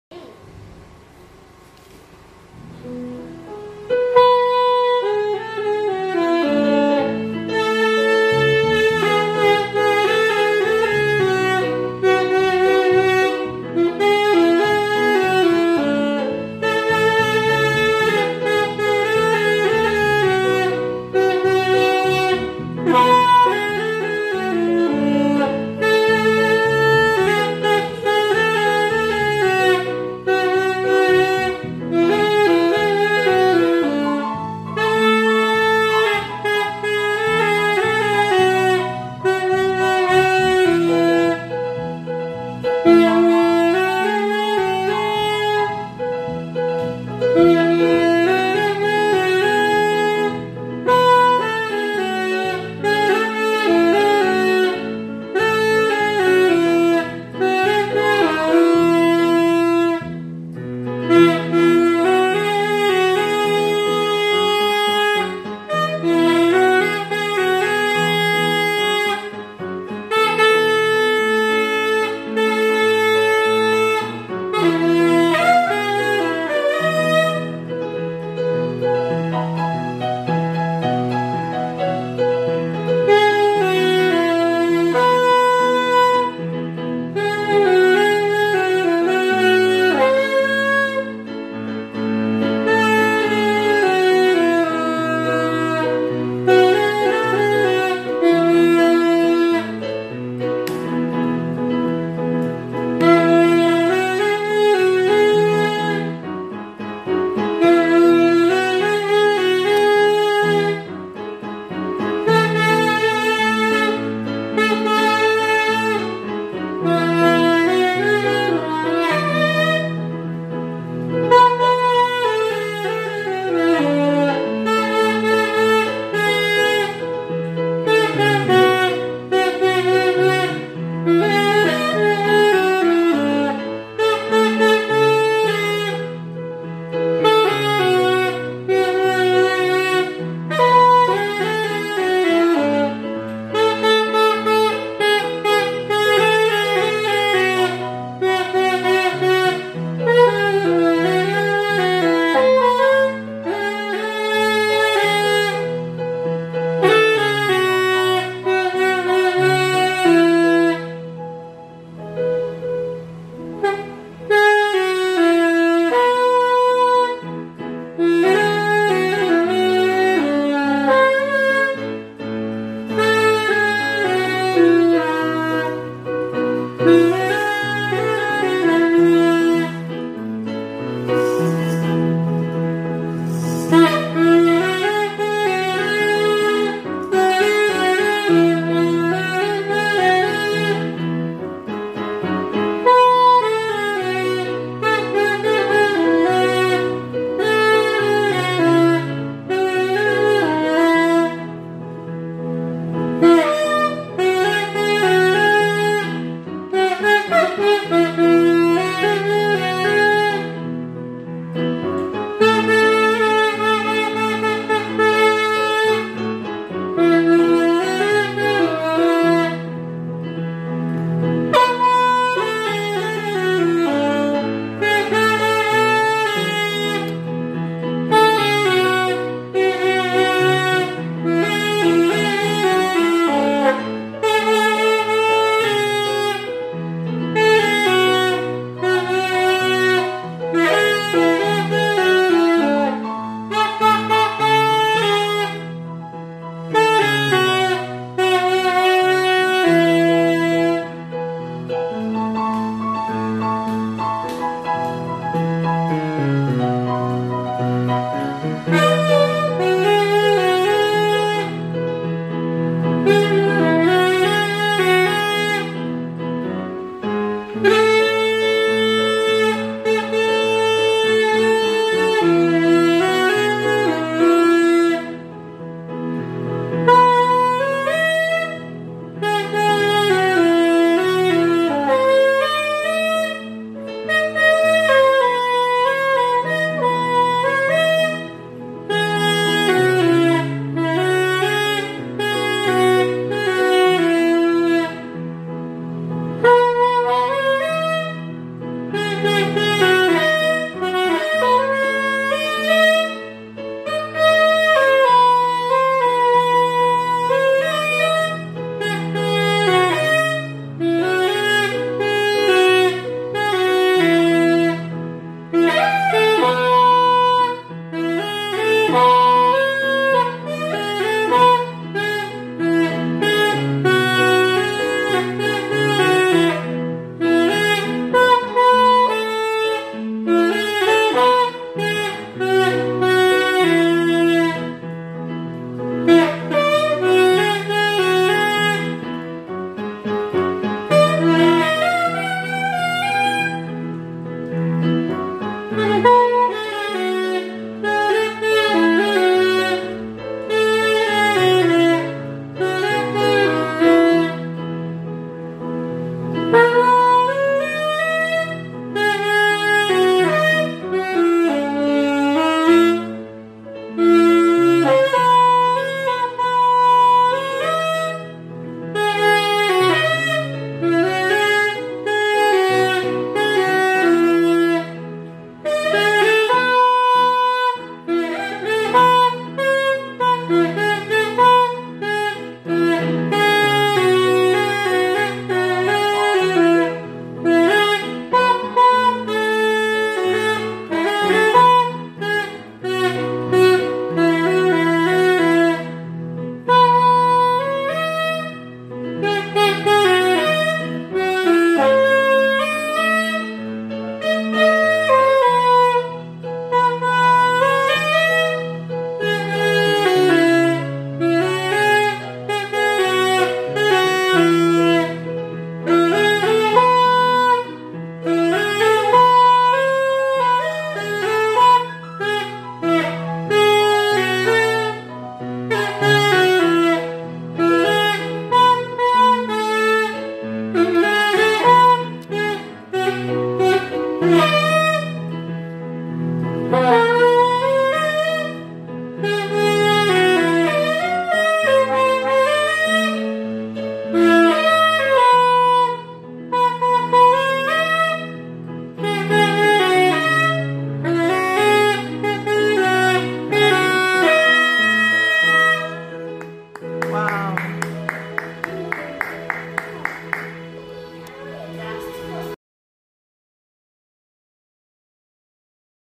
WORSHIP GOD WITH THE YOUNGEST SAXOPHONIST
Oluwadarasimi Onakoya is Six (6) years old. He started learning the saxophone when he was 5+. He is skilled in other musical instruments like the piano, the violin and the recorder. He got involved in musical instruments at age 3. He plays in his school band as well.
He is the youngest Saxophonist we know, he is an inspiration to his siblings, his peers and children all over the world. His favorite subject is mathematics and he loves creative arts. He has very strong moral values and he is a proper child.